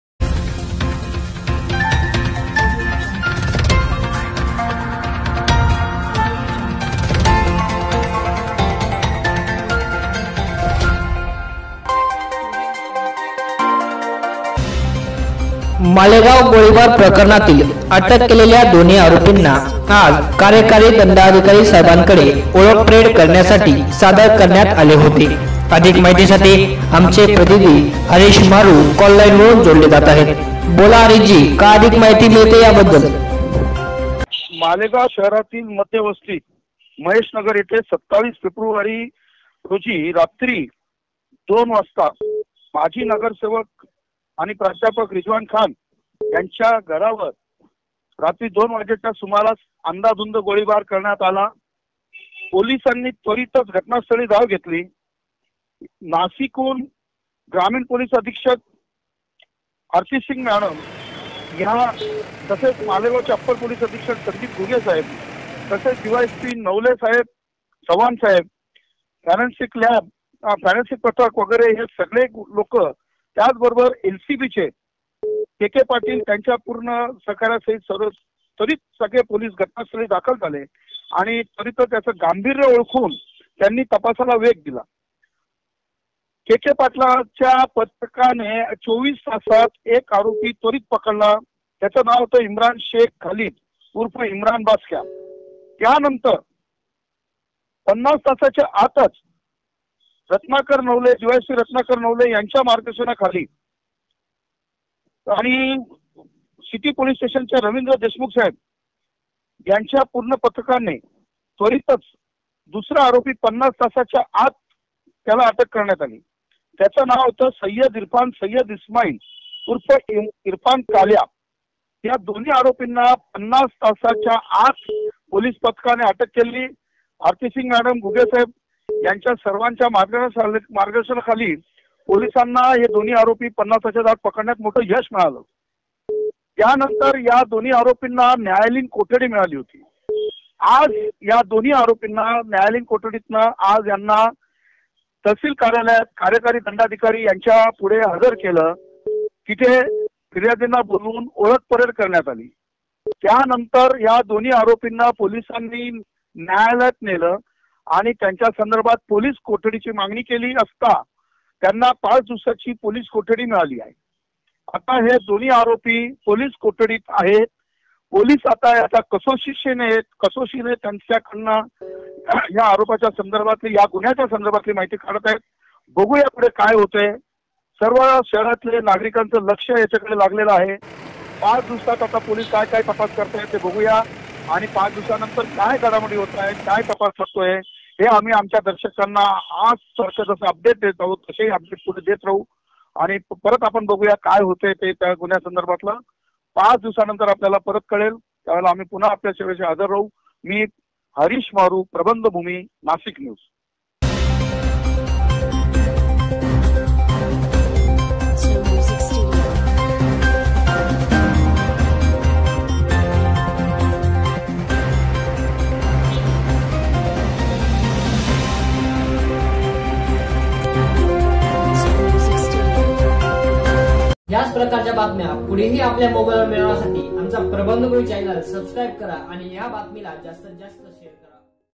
मालेगाव गोळीबार प्रकरणातील दोन्ही आरोपींना न्यायालयाने दिली पाच दिवसाची पोलीस कोठडी
मालेगाव गोळीबार प्रकरणातील दोन्ही आरोपींना
न्यायालयाने दिली पाच दिवसाची पोलीस कोठडी